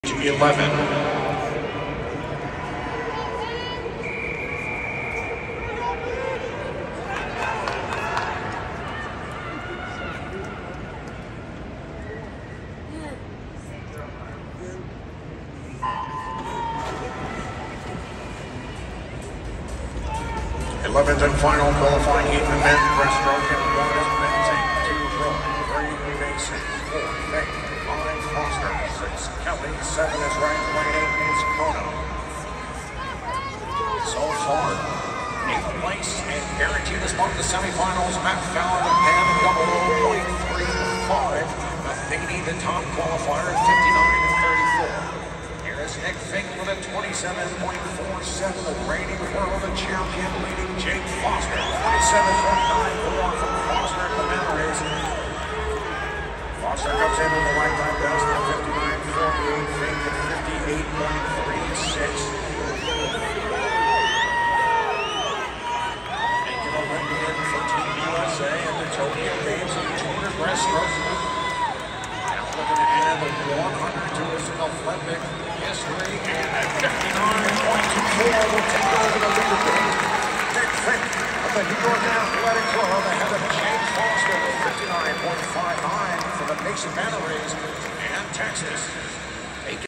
Men's 100m Breaststroke (Heats)_Jake Foster
Men's 100m Breaststroke (Heats)
Jake Foster (Lane 5)
Entry Time:  59.48
Heats Time:  59.59 (+0.11)
Place:  4th
Splits:  27.94 / 31.65